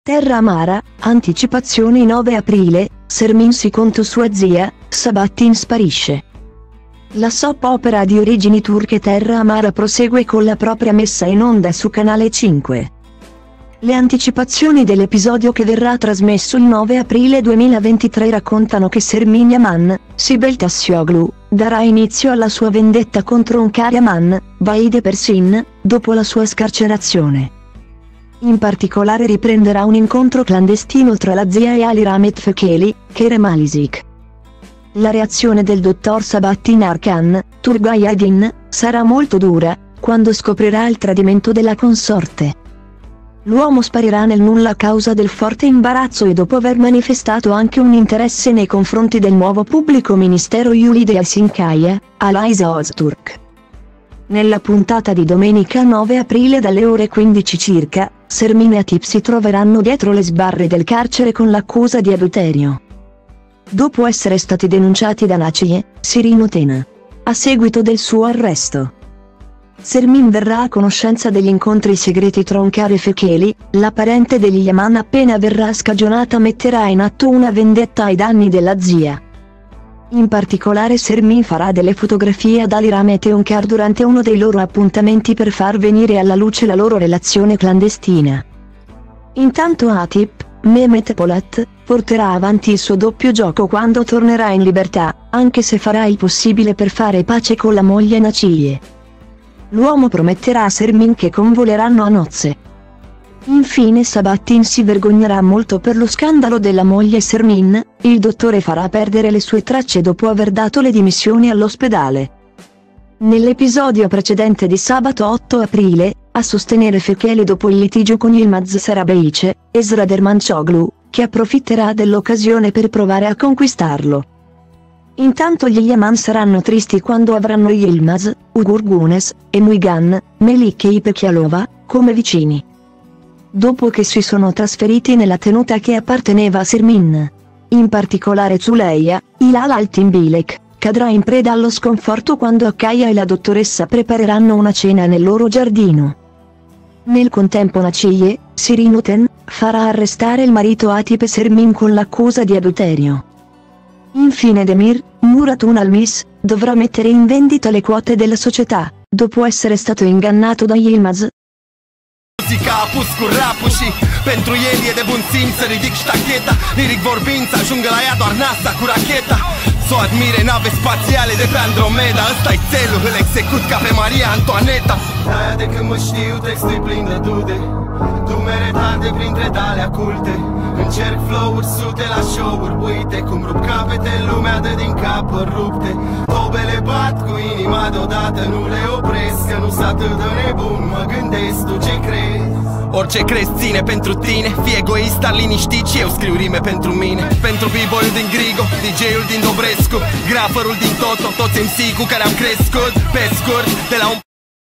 Terra amara,anticipazioni 9 aprile:Sermin si conto sua zia, Sabahattin sparisce
La soap opera di origini turche Terra Amara prosegue con la propria messa in onda su Canale 5. Le anticipazioni dell’episodio che verrà trasmesso il 9 aprile 2023 raccontano che Sermin Yaman (Sibel Tasçioglu) darà inizio alla sua vendetta contro Hunkar Yaman (Vahide Perçin) dopo la sua scarcerazione,in particolare riprenderà un incontro clandestino tra la zia e Ali Rahmet Fekeli (Kerem Alisik).